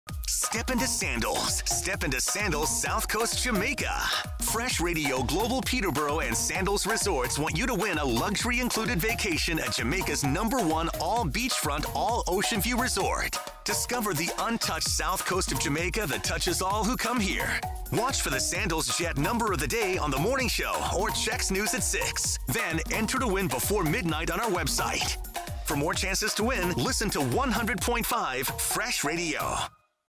Step Into Sandals and Win Your Way to Jamaica - V1
Step Into Sandals with 100.5 Fresh Radio and Global Peterborough.  Contest runs January 7th to February 1st, 2019.